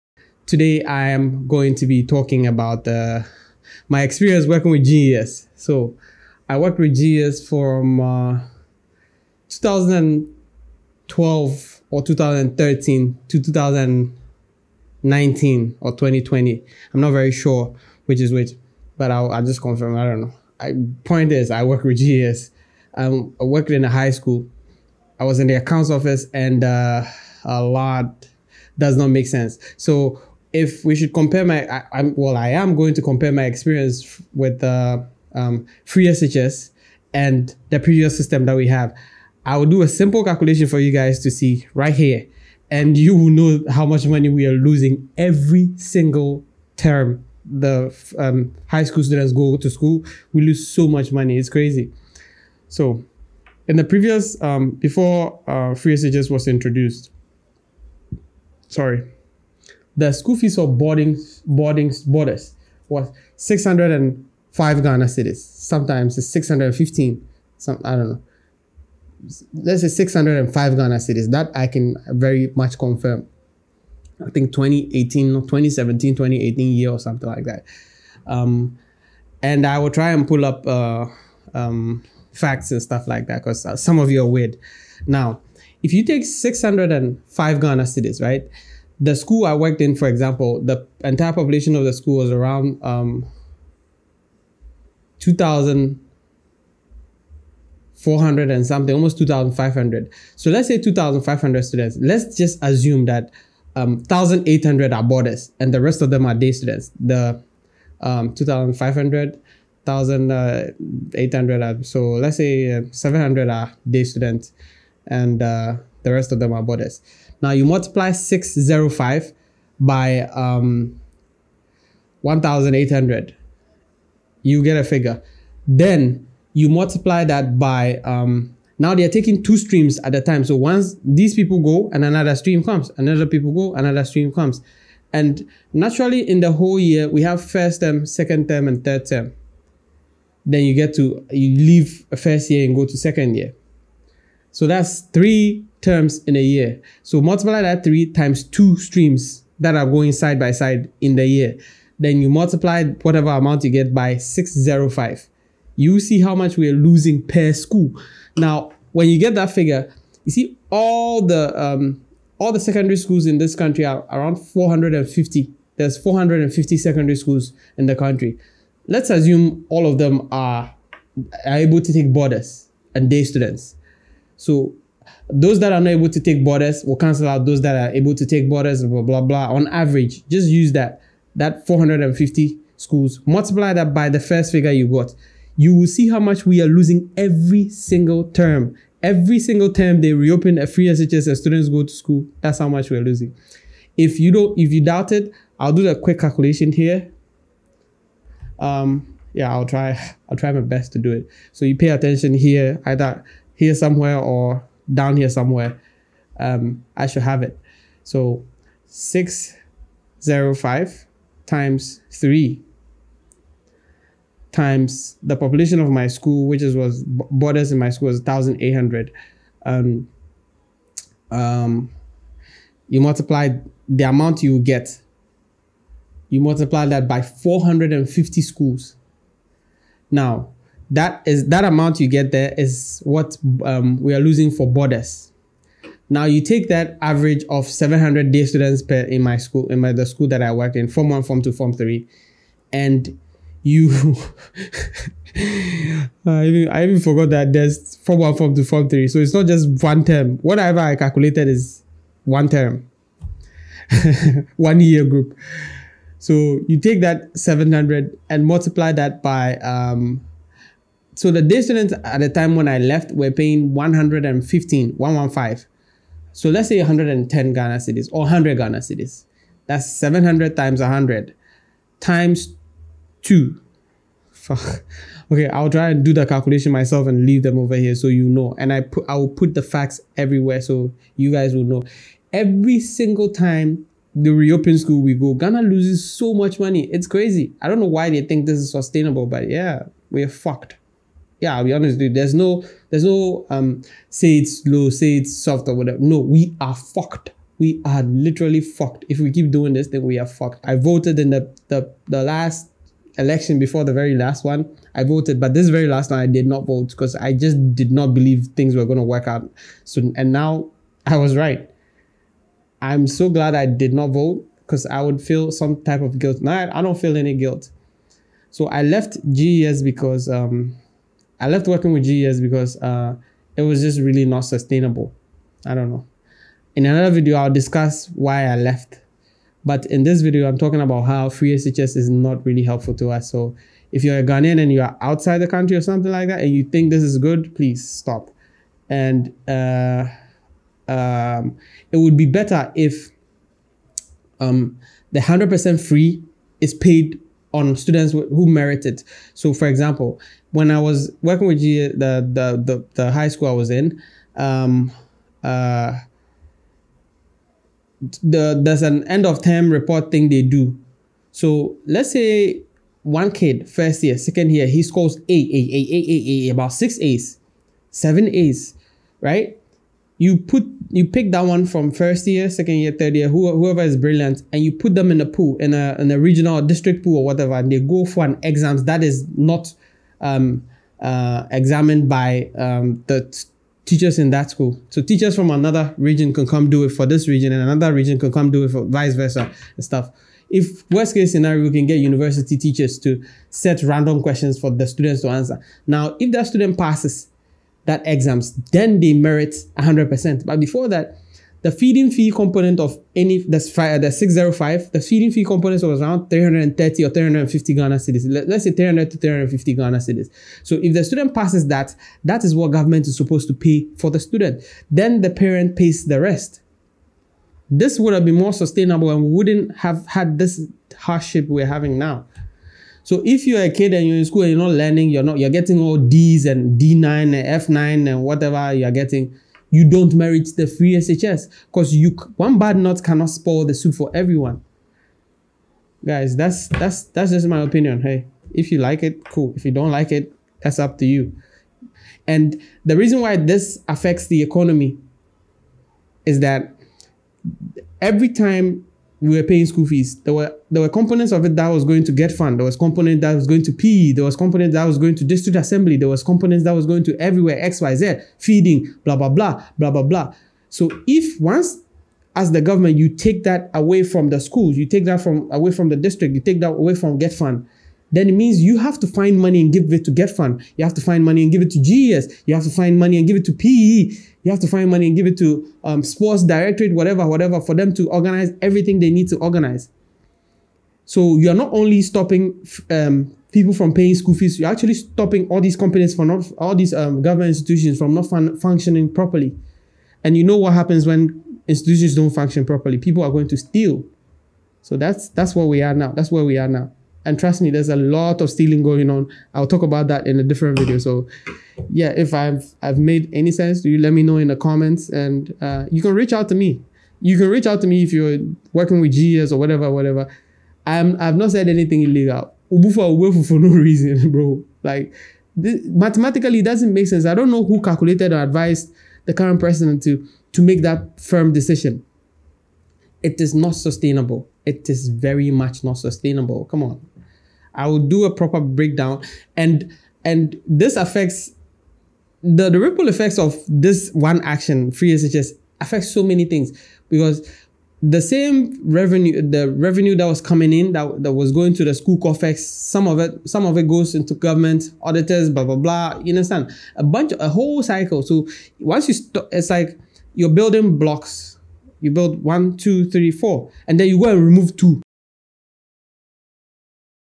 GHANA's FREE SHS 🇬🇭 COLLAPSING THE ECONOMY • My H O N E S T opinion 🤬
I was a Ghana Education Service employee for a couple years. I left voluntarily after I found a flaw in the cash receipt system, reported to my superior but was made to feel as if I wanted to steal instead lol. So I wrote my resignation letter and didn't even wait for a reply & left.

Here's my honest take on FREE SHS